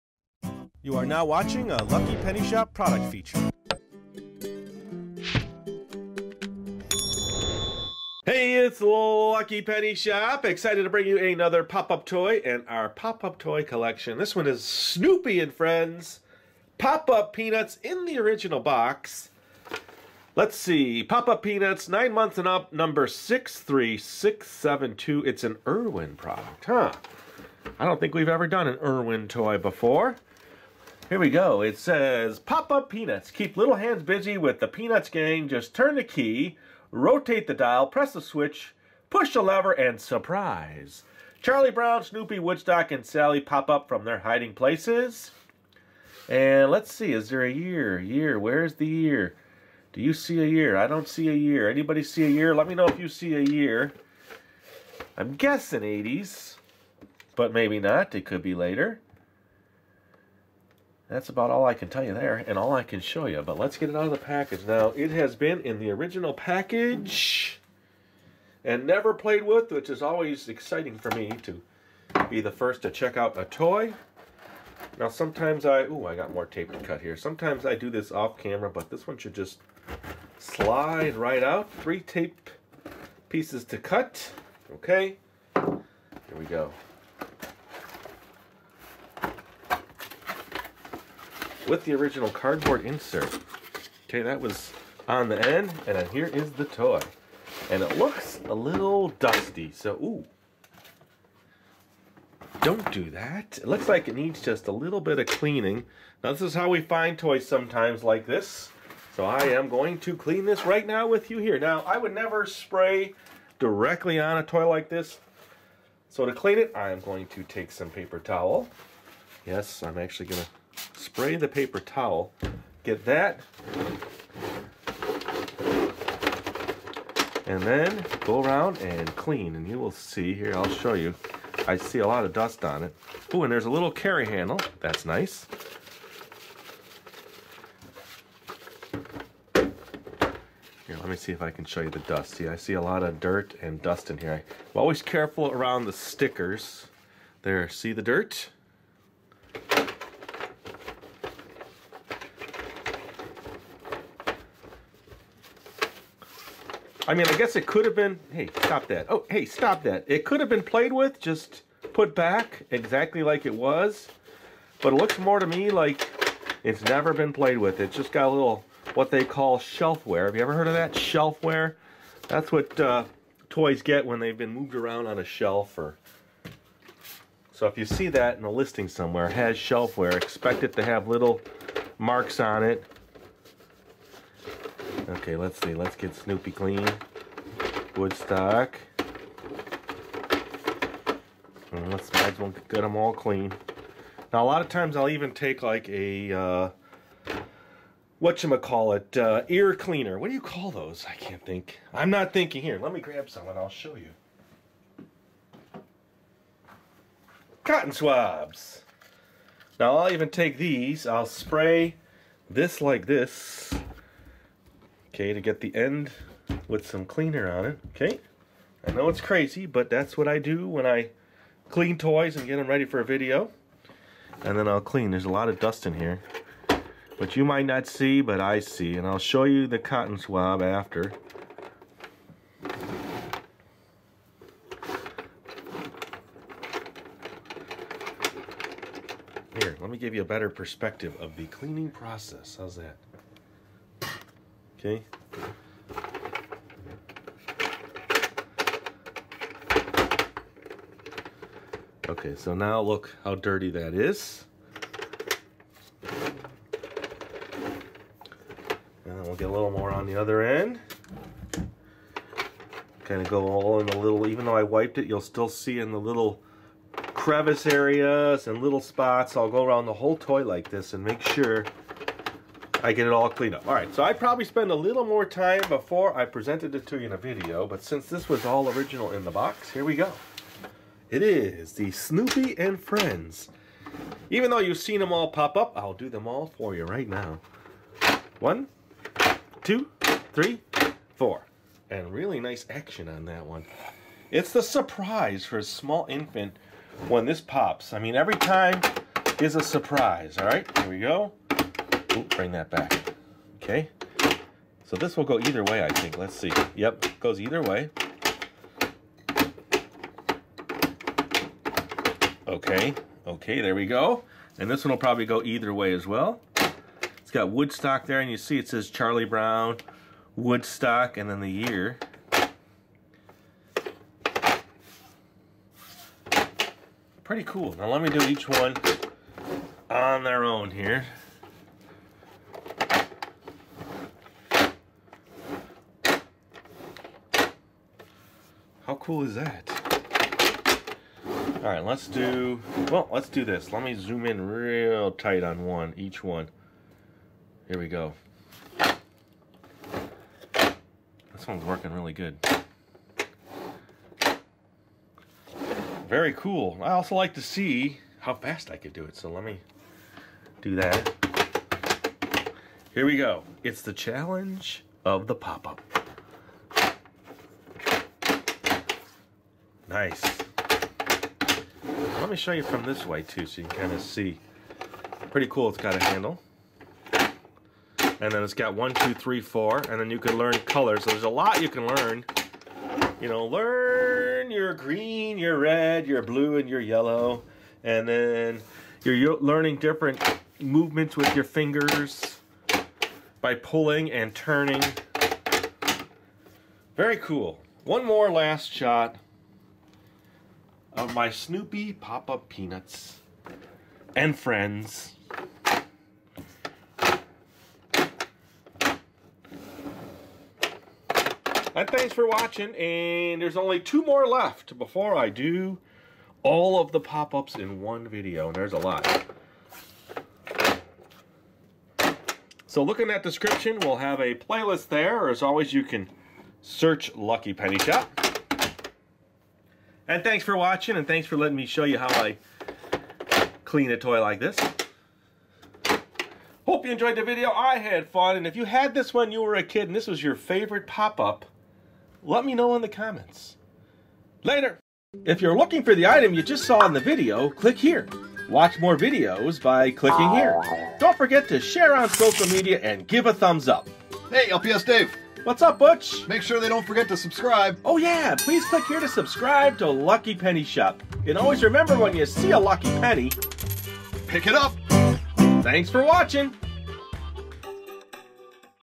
Snoopy & Friends Pop-up Peanuts Kids Poppin' Pals Toy With Charlie Brown, Woodstock & Sally
Snoopy & Friends Pop-up Peanuts!  Almost ready to show the complete collection of Poppin' Pals.  Check out Snoopy & Friends!

Lucky Penny Thoughts: I hope it was OK to clean this on video.  I like showing some of the steps it takes to get an item ready for a video.  It was a surprise to find the Snoopy & Friends Pop-up Peanuts from Irwin  #63672 in the original box!

Later!
LPS-Dave

▶▶ Product Info
Snoopy & Friends Pop-up Peanuts -  Irwin Toys